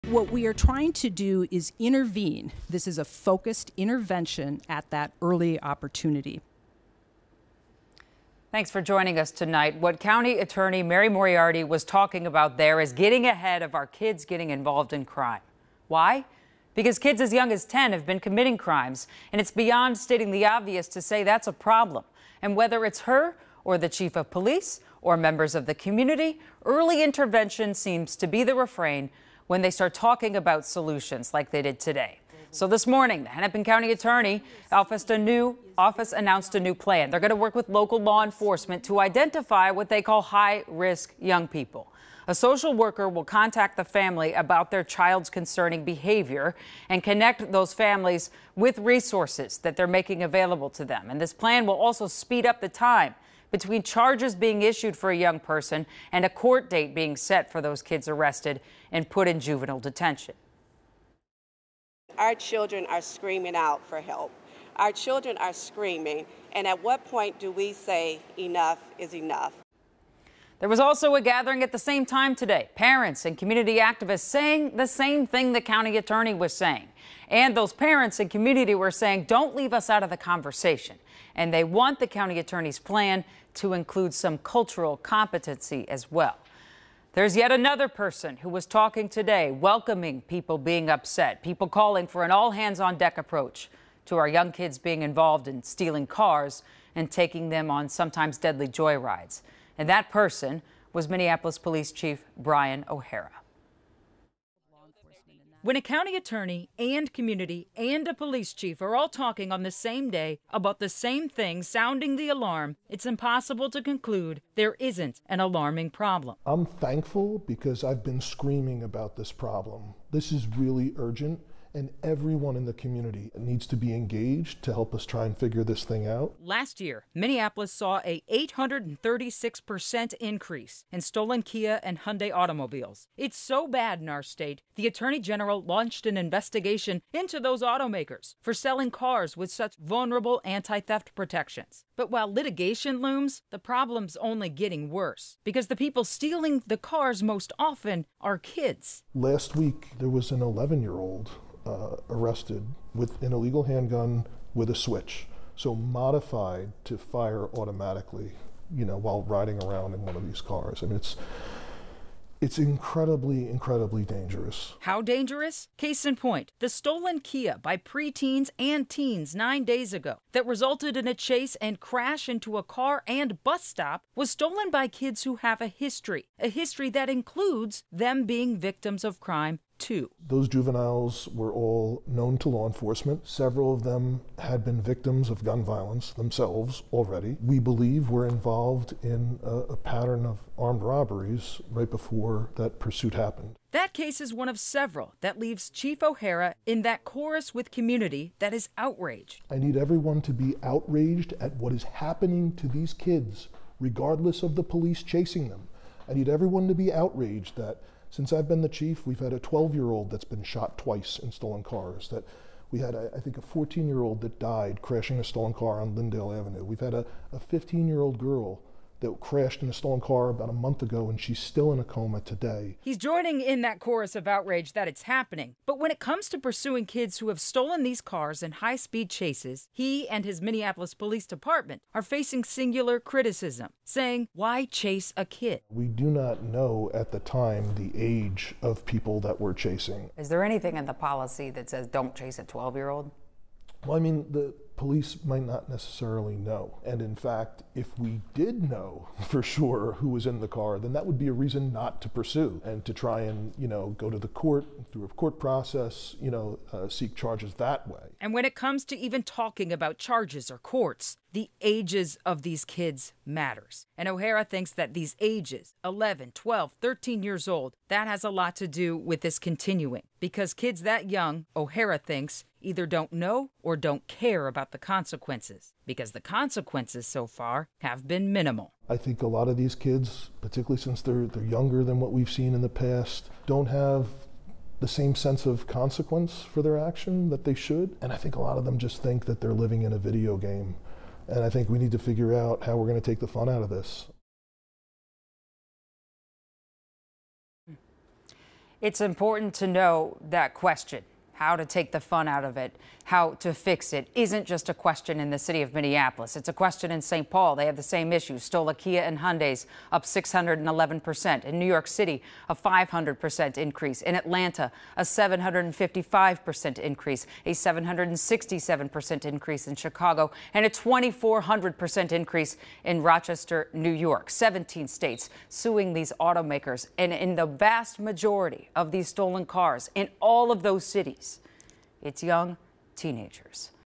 MPD Chief Brian O'Hara discusses juvenile crime
The chief sat down with KARE 11's Jana Shortal to discuss a wave in youth crime.